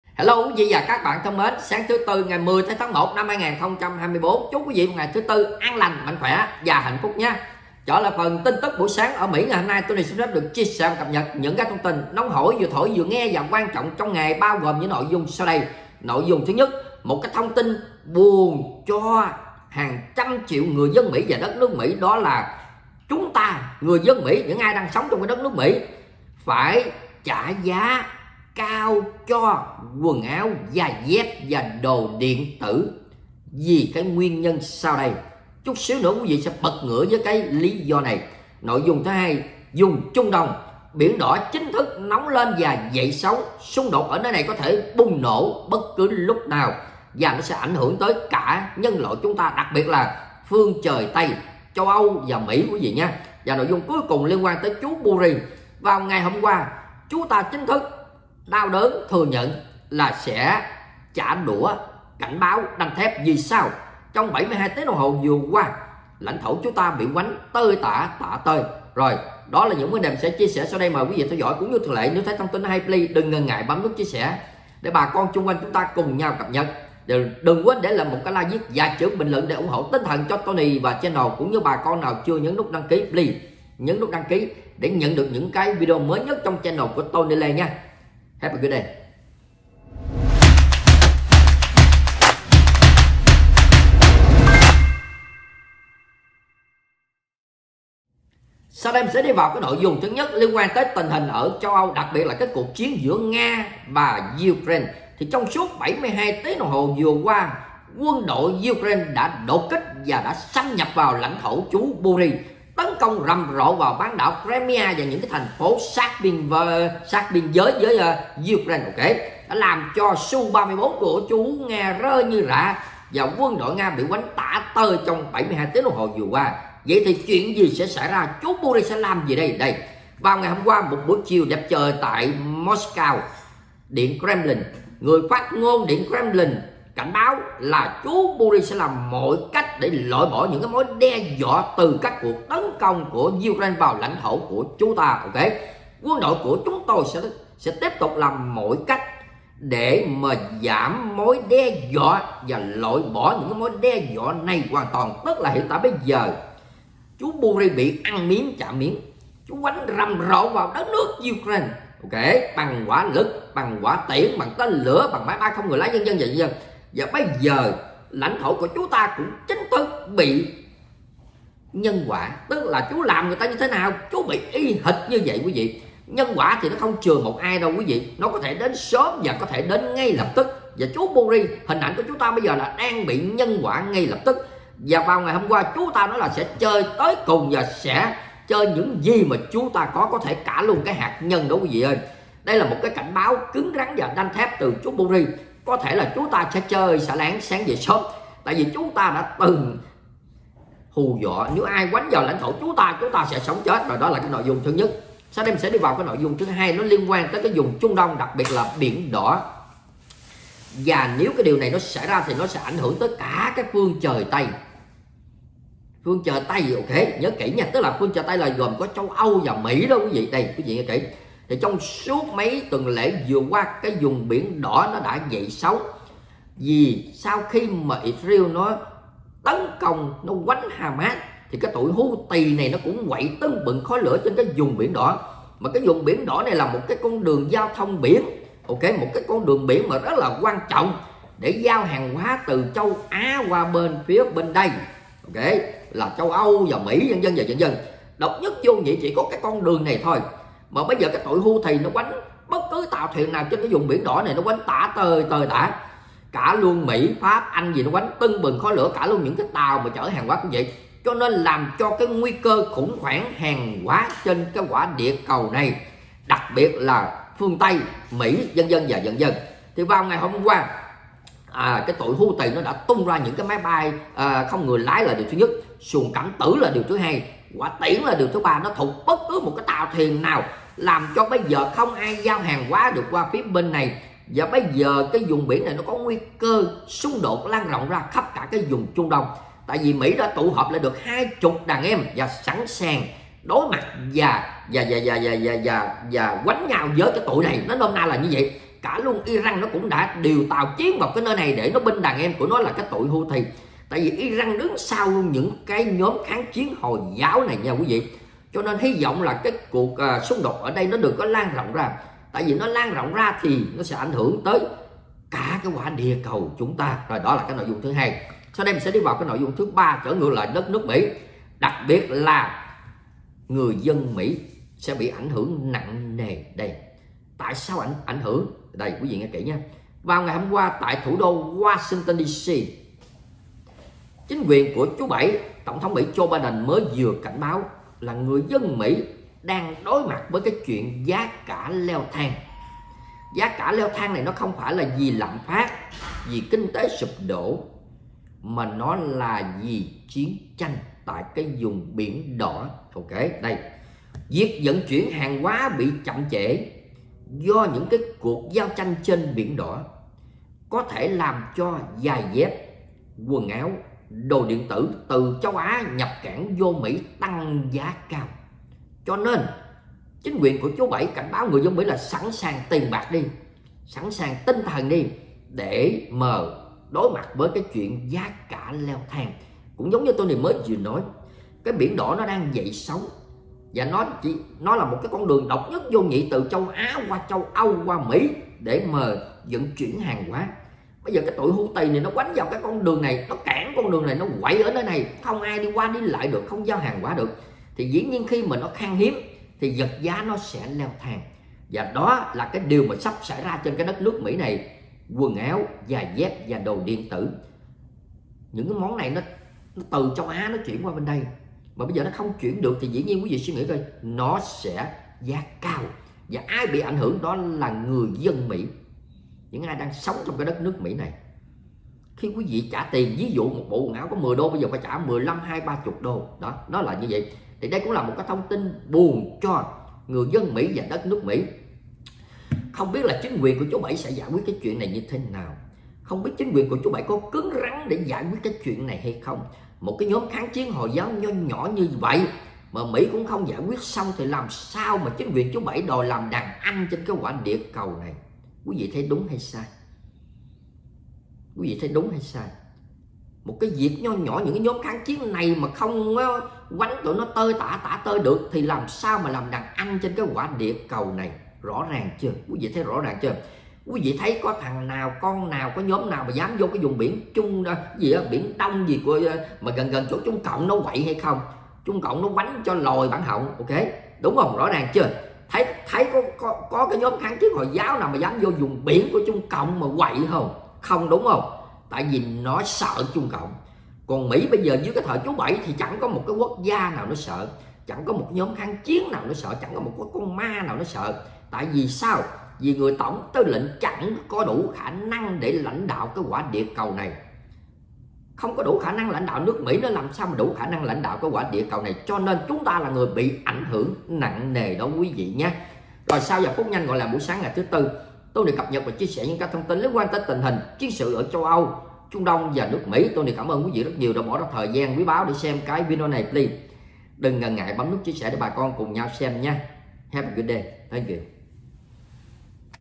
TIN NÓNG | TRUNG ĐÔNG Biển Đỏ nóng lên: Xung đột có thể bùng nổ và lan rộng ....
With LOVE,
Tony Lê from Texas, USA 🇺🇸